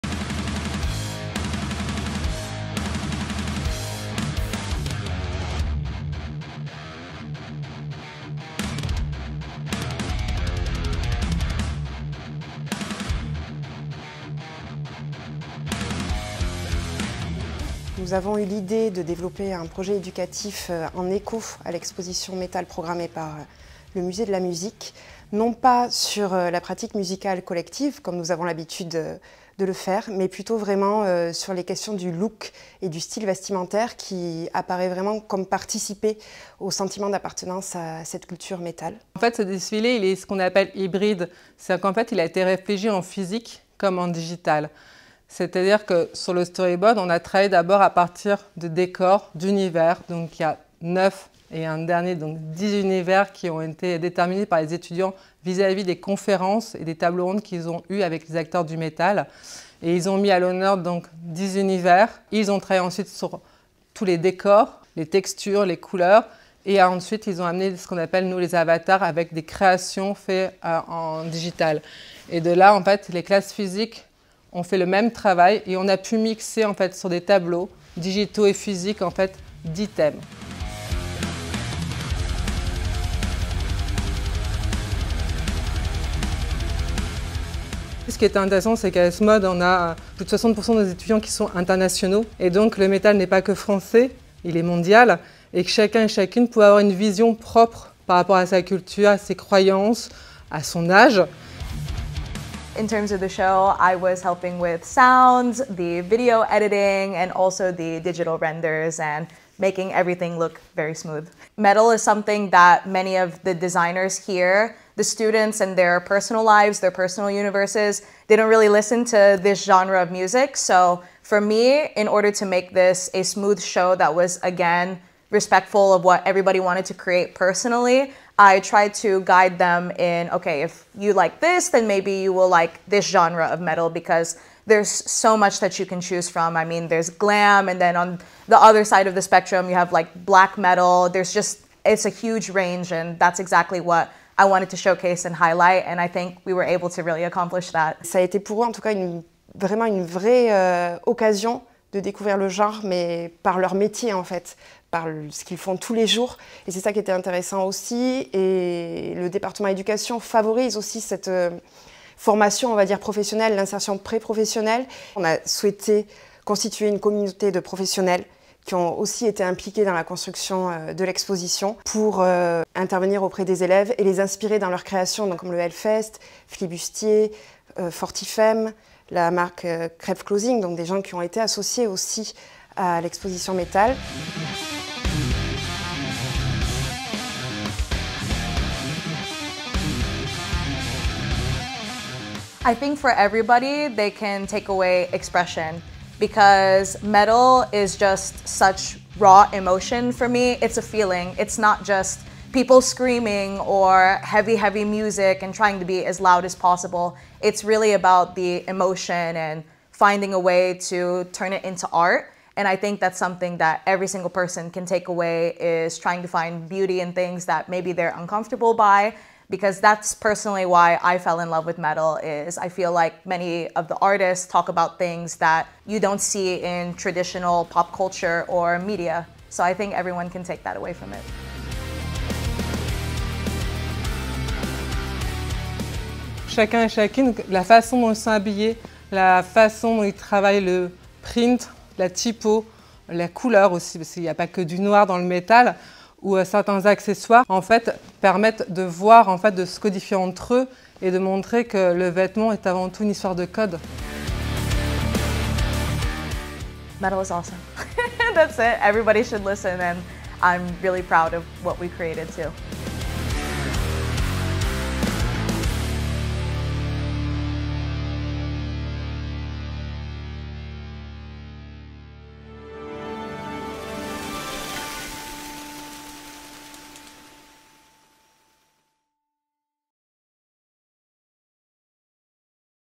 Retour sur... Défilé Metal | ESMOD
En avril 2024, le Département Éducation de la Philharmonie de Paris s'associait à l’École supérieure des arts et techniques de la mode (ESMOD) pour une création originale dans le cadre de la programmation dédiée au metal.

✏️ Entretien : Guillaume Dubigny
🎥 Réalisation & montage : Laurent Sarazin, Clément Gaultier-Falguière - Imaginé productions